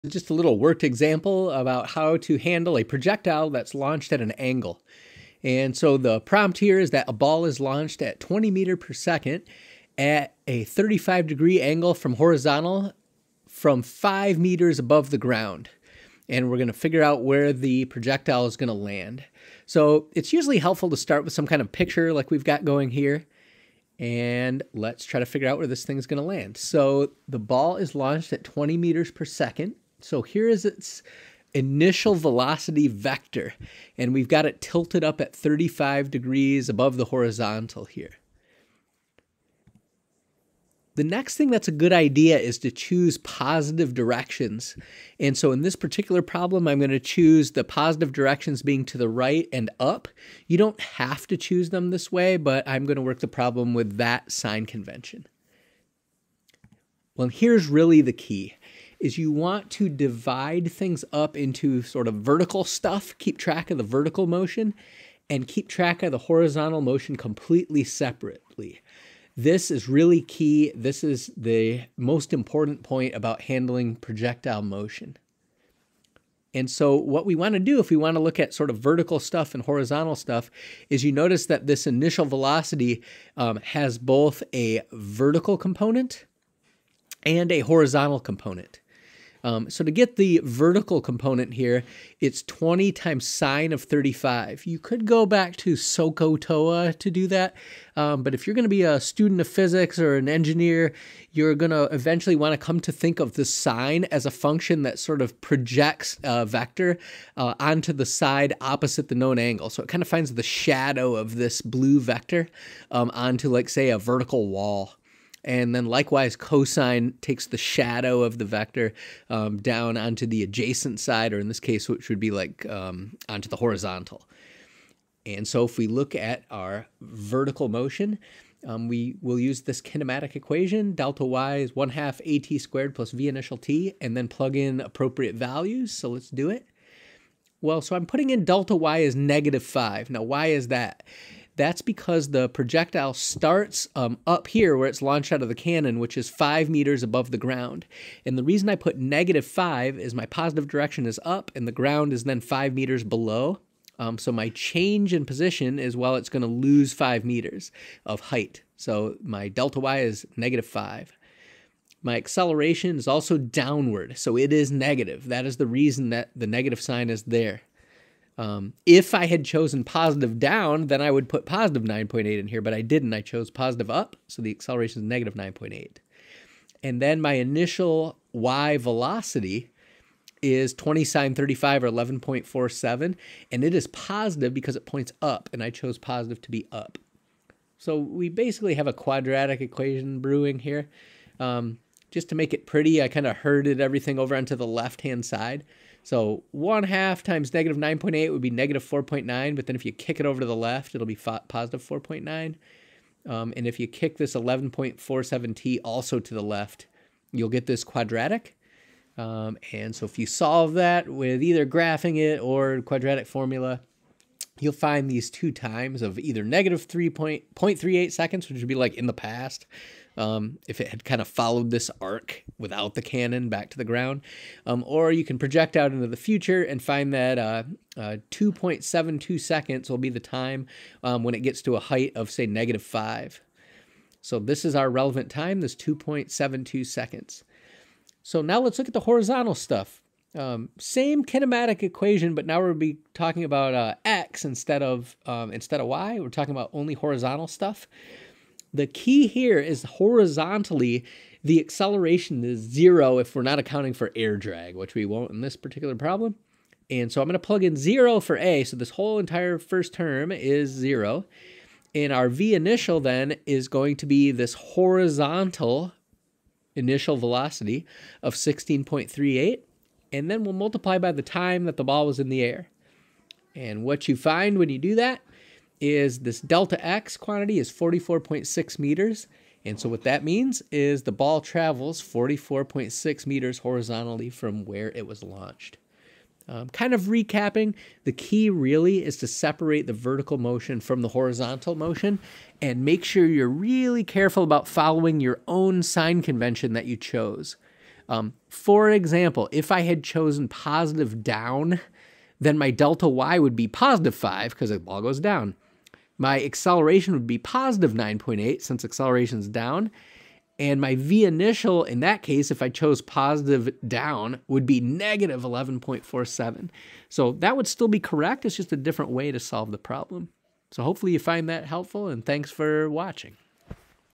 Projectile Motion & Kinematics: How to Handle Projectiles Launched at an Angle
We solve for the horizontal range of a projectile launched at an angle from a cliff, using kinematic equations for constant acceleration. The key is to completely separate the vertical motion and the horizontal motion.  In the absence of air friction (air drag) the vertical acceleration is 9.8 (m/s)/s downward and the horizontal acceleration is ZERO.  The initial vertical velocity is the vertical component of the velocity vector.  You can use trigonometry to find it; it is v0 multiplied by the sine of the angle above horizontal. Likewise, the initial horizontal velocity is the horizontal component of the velocity vector.  It is typically v0 multiplied by the cosine of the angle above horizontal.  A useful strategy is to use the vertical information to solve for the time of flight with a kinematic equation and then use the distance = rate x time in the horizontal direction to find the horizontal range.  Again, the keys to success are to treat the horizontal and vertical motions independently and to follow sign conventions that are consistent with your choices of positive and negative directions.